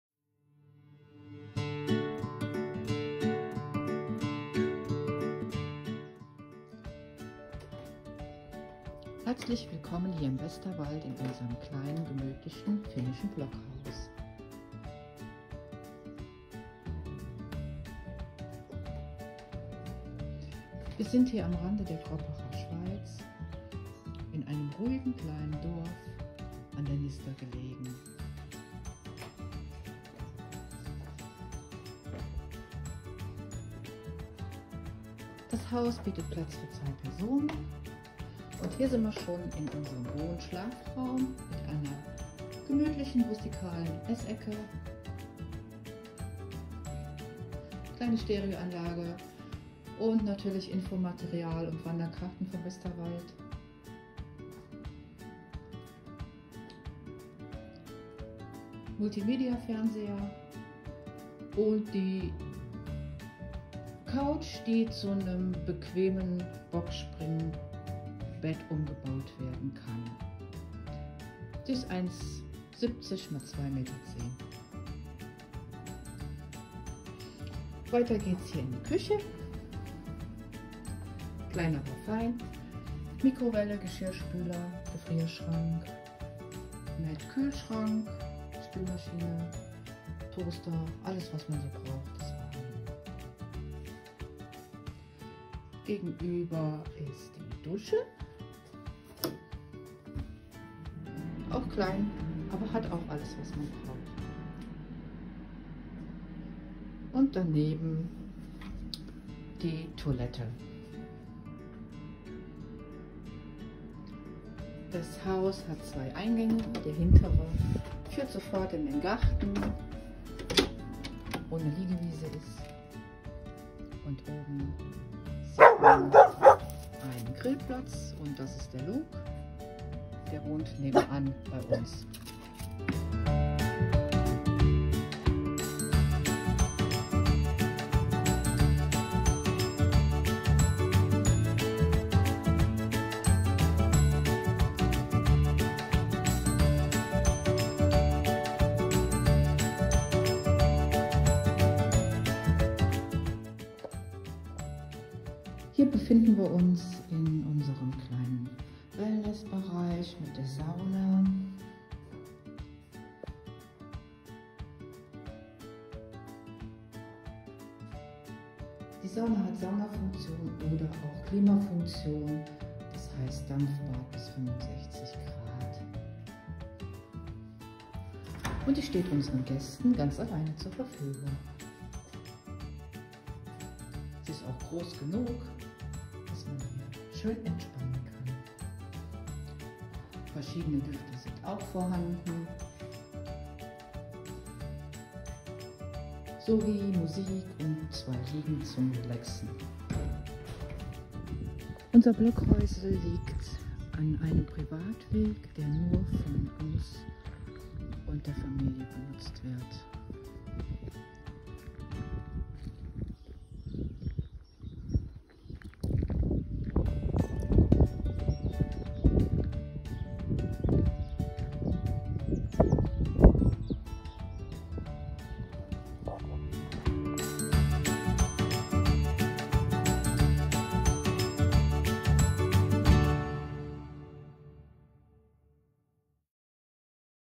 Rundgang: Helmerother Blockhäusl - Freistehendes Blockhaus mit tollem Ausblick in den Westerwald
Freistehendes gemütliches Blockhaus waldnah am Südhang gelegen, mit einem großem Balkon und sehr schönen Ausblick in die Kroppacher Schweiz mitten im Westerwald. Urlaub, Erholung in Ruhe und Natur im schönen Nistertal.

Durch die massive Blockbohlenbauweise bietet das Haus ein wunderbares Raumklima, es ist liebevoll und gut eingerichtet. Im Wohnschlafbereich befindet sich ein hochwertiges Schlafsofa, das mit nur 2 Handgriffen zu einem bequemen Bockspringbett mit viscoelastischem Topper umgewandelt werden kann.

Das Haus wird mit Fußbodenheizung beheizt (überall Steinzeugböden), im Bad befindet sich ein zusätzlicher Handtuchheizkörper. Wlan und ein 40 Zoll Smart TV sind vorhanden.

Ab Mitte Oktober 2020 befindet sich im Erdgeschoss, das über die Außentreppe des Blockhauses erreichbar ist, ein kleiner Wellnessbereich mit großzügiger Sauna/ Dampfbad. Dieser kann auf Wunsch, natürlich nur zur alleinigen Nutzung, zugebucht werden.

Umgebung vom Helmerother Blockhäusl

Helmeroth liegt an der Nister, umrahmt von Hügeln und Wäldern. Der ideale Ausgangspunkt für schöne Wanderungen, der Westerwaldsteig ist ganz nah. Die Kreisstadt Altenkirchen ist 10 km entfernt, Einkaufsmöglichkeiten findet man im Nachbarort (Bäcker und Metzger) und in den nächst größerern Orten Hamm (6 km) und Wissen (8 km). Frei - und Hallenbäder in 6 - 8 km Entfernung. Kletter- und Tierpark 18 km, Baumwipfelpfad 20 km, Angelmöglichkeit 8 km, Kartbahn 4 km.